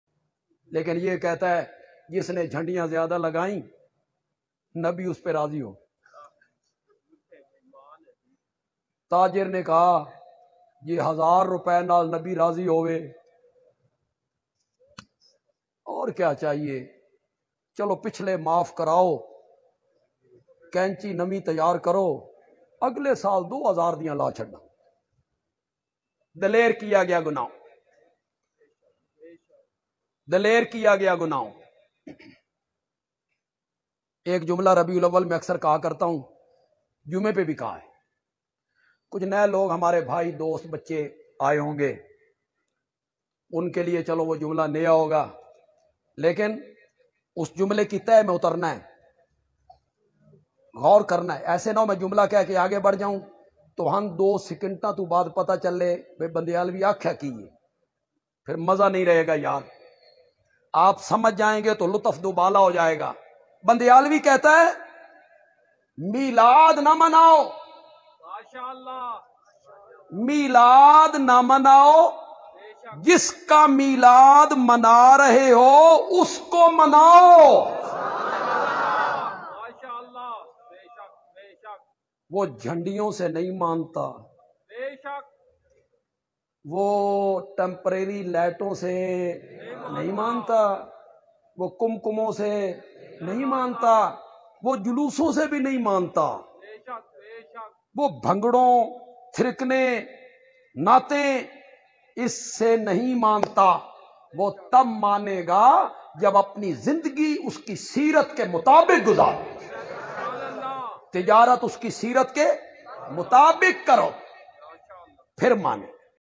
Nabi (SAW) Milad Ki Lighting Say Nahi Manty - Allama Attaullah Bandyalvi
نبی صلی الله علیہ وسلم لائٹنگ سے نہیں مانیں گے۔۔!!